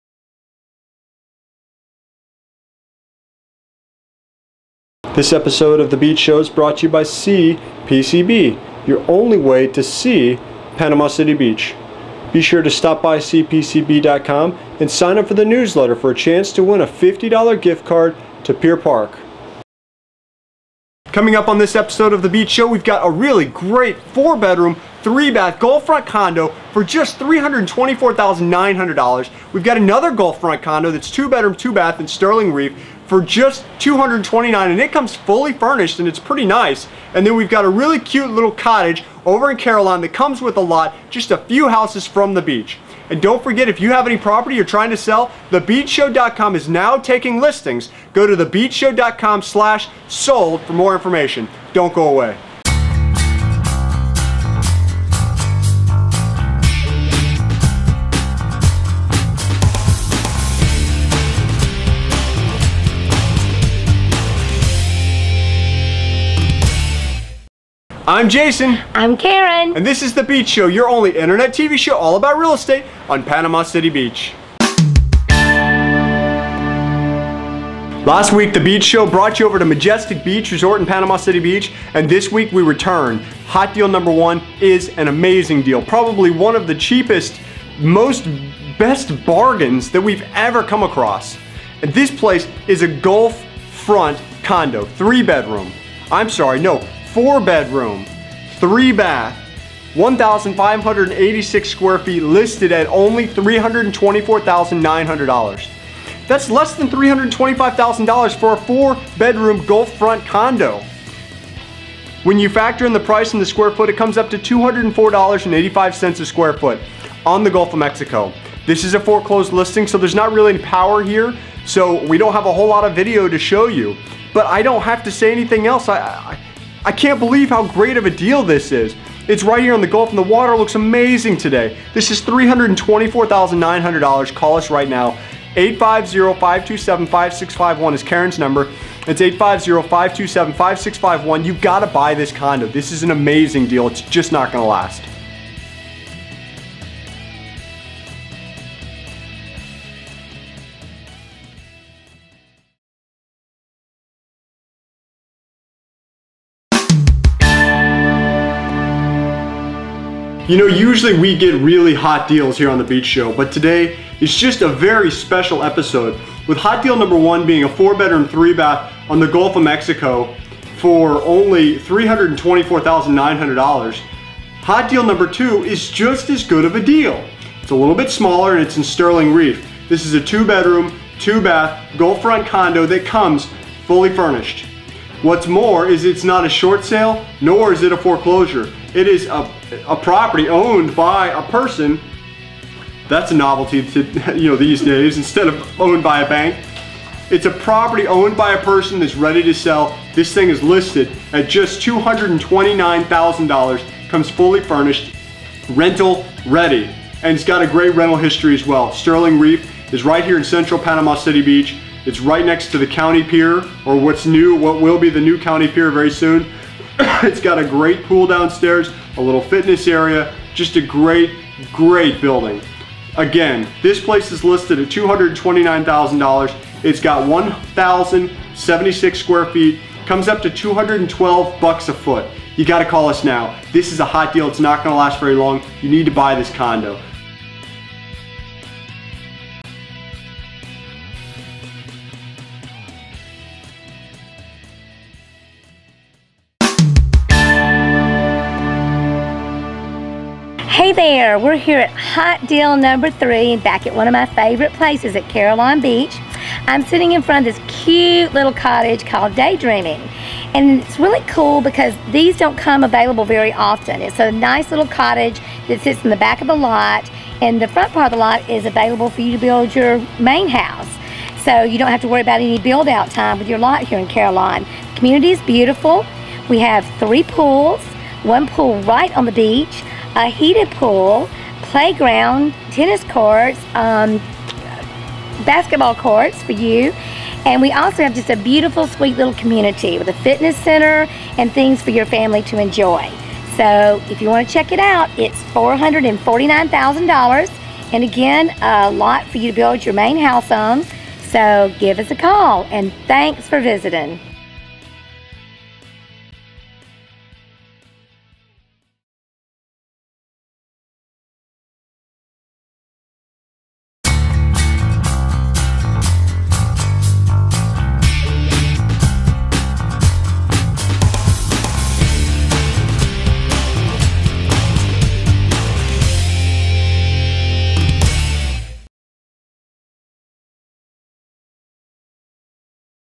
Episode #21 - Gulf-front 4 Bedroom Condo $324,900
# Deal 1 - Gulf-front 4 Bedroom Condo $324,900
# Deal 2 - 2 Bed 2 Bath Fully furnished Gulf-front Condo for only $229k
# Deal 3 - Cute Cottage in Carillon WITH LOT steps from the private beach